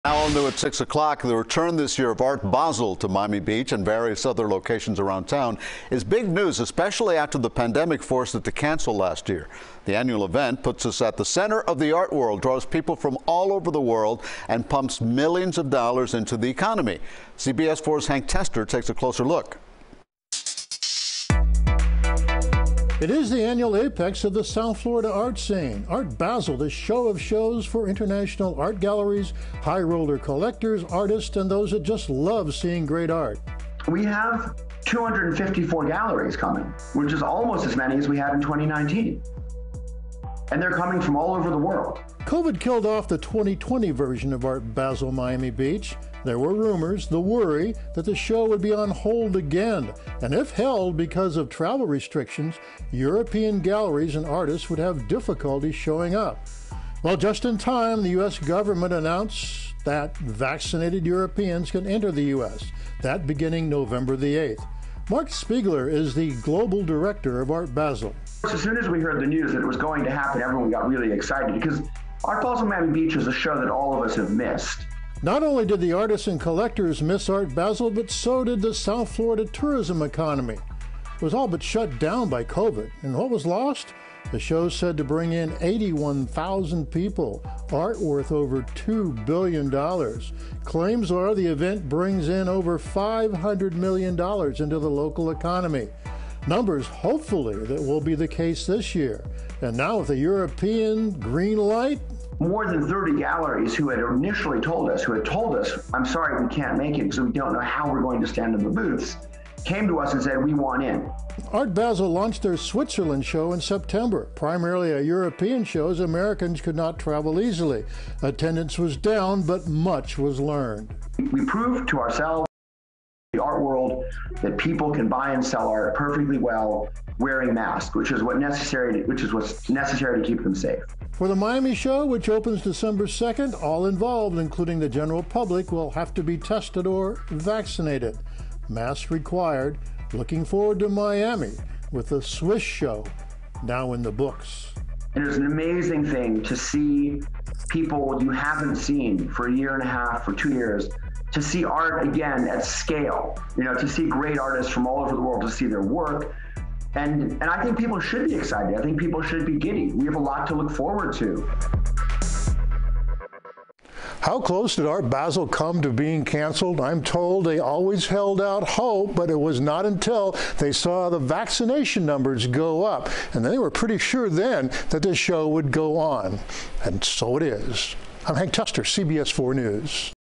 Art Basel Is Back In Full Force
Art Basel returns with 254 galleries from all over the world